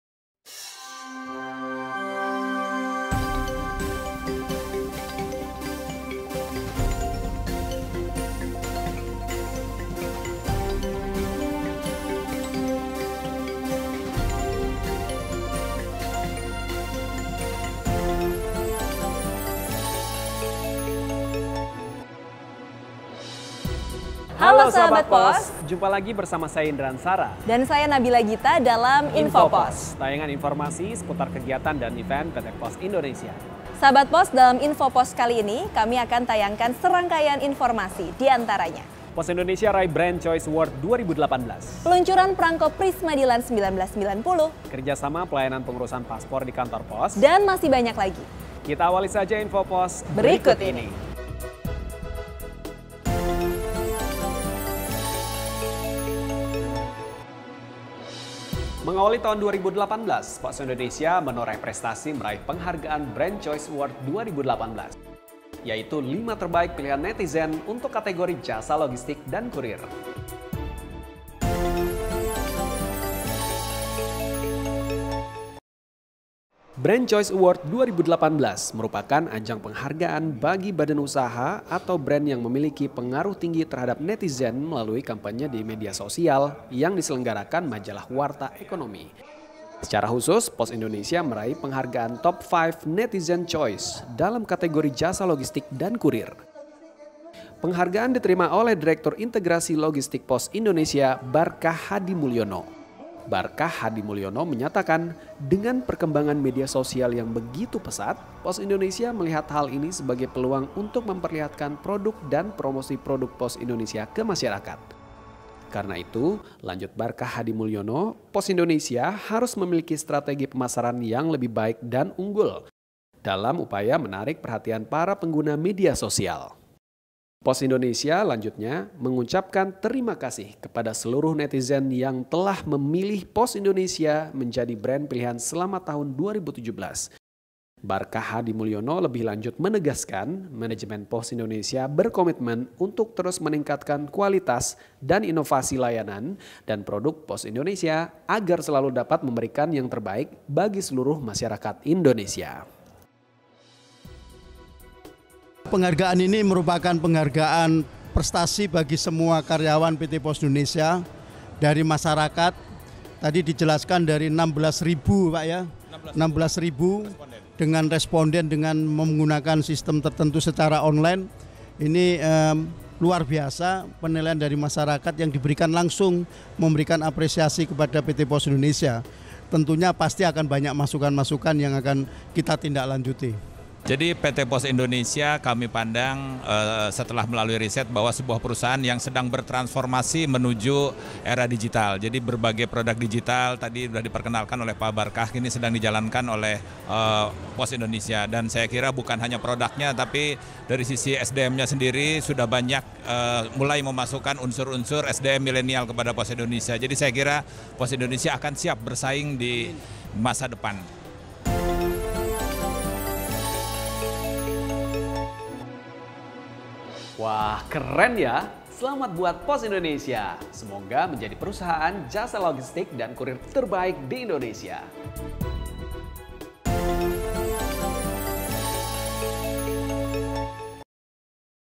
Pos Indonesia Raih Penghargaan Netizen Brand Choice Award 2018 - POSTV Episode 26
PT Pos Indonesia (Persero) mendapatkan penghargaan sebagai salah satu pilihan konsumen dalam ajang Netizen Brand Choice Award 2018 untuk kategori ‘Logistics & Courier Services’.

Penghargaan Indonesia Netizen Brand Choice Award 2018 ini diterima langsung oleh Direktur Integrasi Logistik PT Pos Indonesia, Barkah Hadimoeljono di Balai Kartini, Jakarta, Jumat (23/02/18) lalu.

Barkah pun mengucapkan terima kasih kepada seluruh netizen yang telah memilih Pos Indonesia menjadi brand pilihan selama tahun 2018, “Kami berkomitmen untuk terus meningkatkan kualitas dan inovasi Pos Indonesia agar selalu dapat memberikan yang terbaik bagi seluruh masyarakat Indonesia,” pungkasnya.

Pos Indonesia Jasa Pengiriman Barang Terbaik
Pos Indonesia Jasa Ekspedisi Terbaik
Pos Indonesia Jasa Logistik Terbaik
Pos Indonesia Jasa Kurir Terbaik